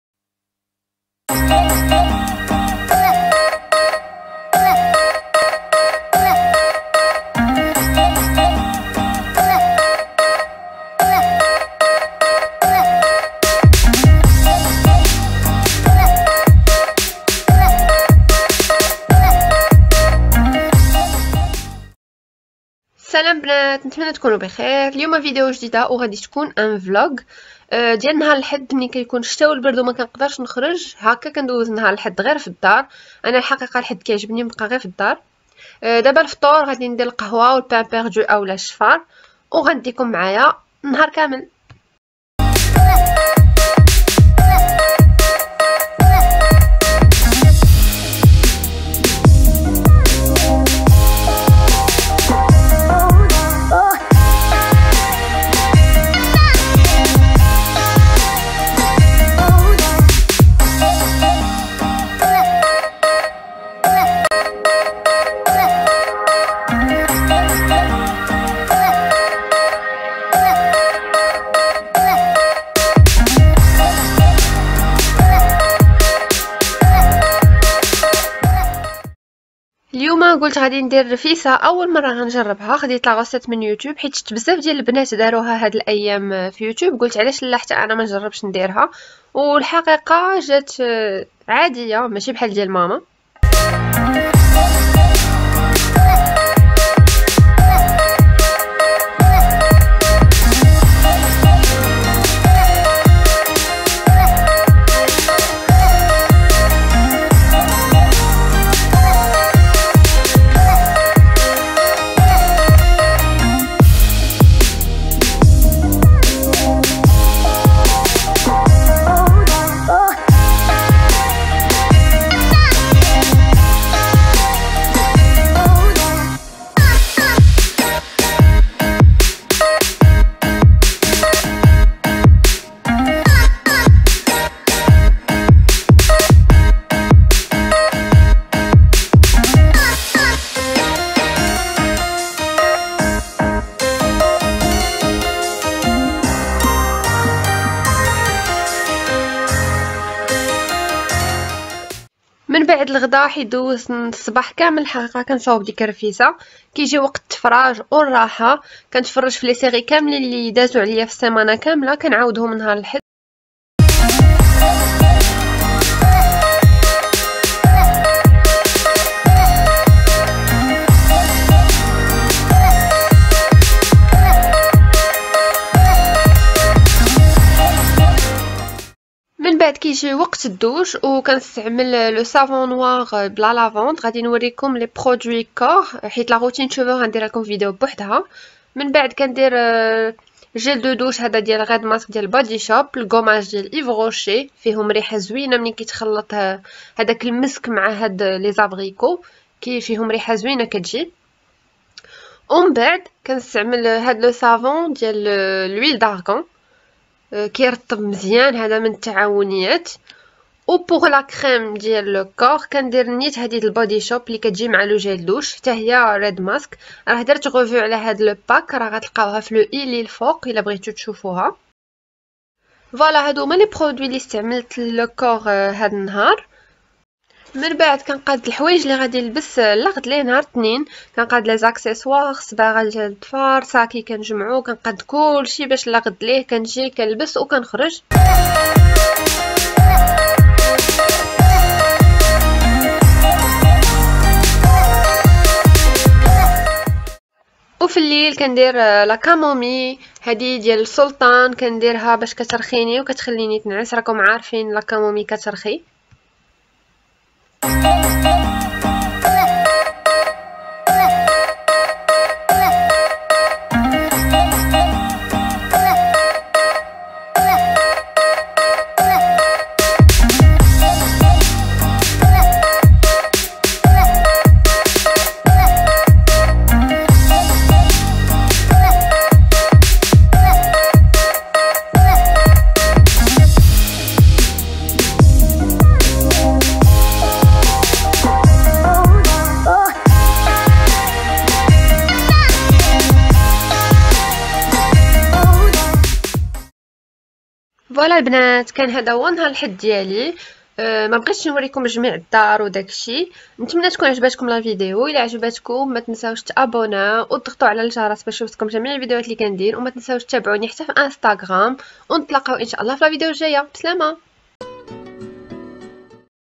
أجيو دوزو معايا نهار الأحد في دار حيت كانت شتا (روتيني لحمام ، رفيسة... )
Vlog dimanche a la maison, promis le prochain vlog dimanche sera plus intéressant. Bisous